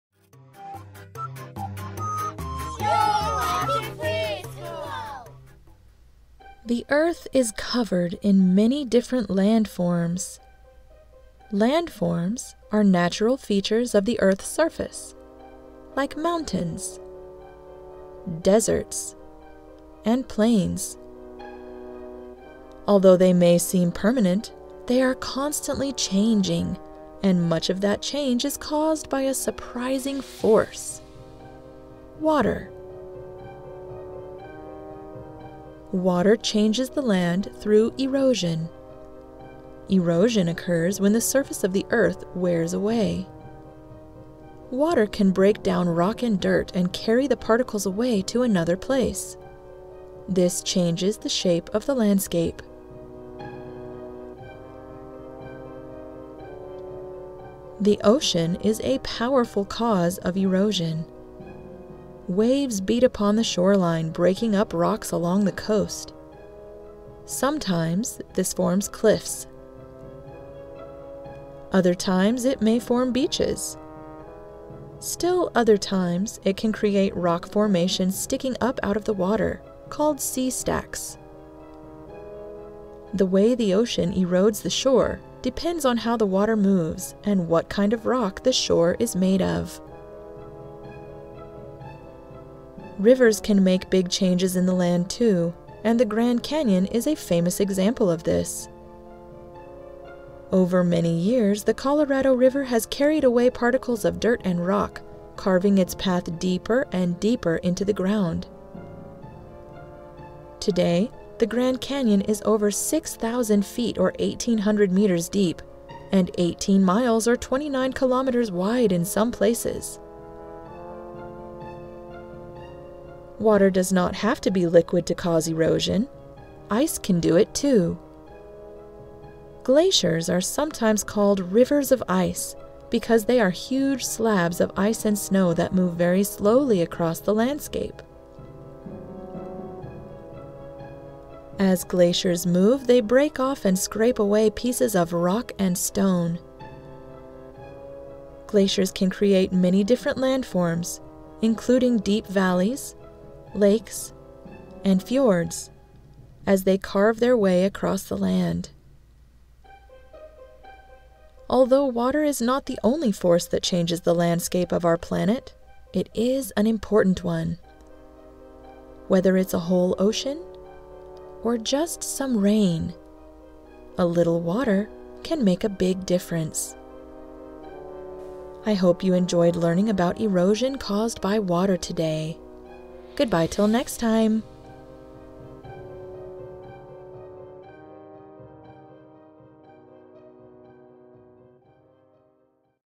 The Power of Water for Kids: How Erosion by Water Shapes Landforms for Children - FreeSchool
- Help support more content like this!
The landscape around us may look permanent, but it is really changing all the time. One force that changes the shape of our planet may surprise you: it's water! Water can change landforms through erosion, the process by which the Earth's surface is gradually worn down. Come and learn about it in this fun and educational video!

Like this video if you want to see more videos about EARTH SCIENCE!